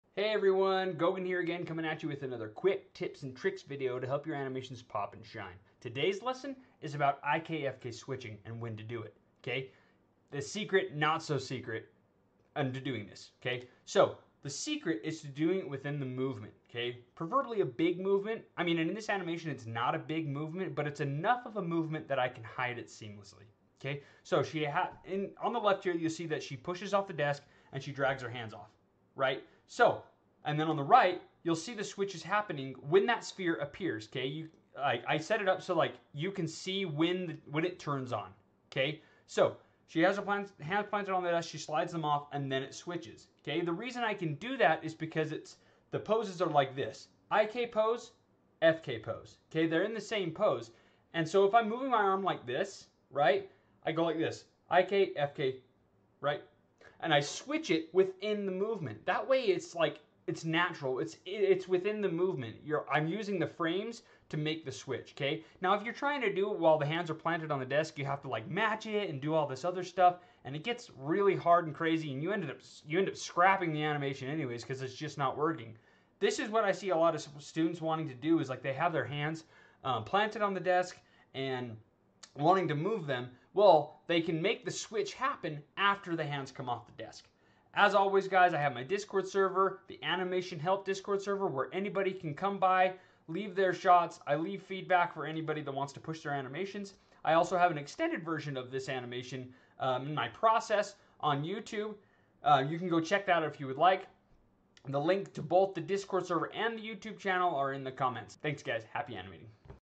IKFK Switching Quick Trick
This video gives a quick trick on how to seamlessly switch from IK to FK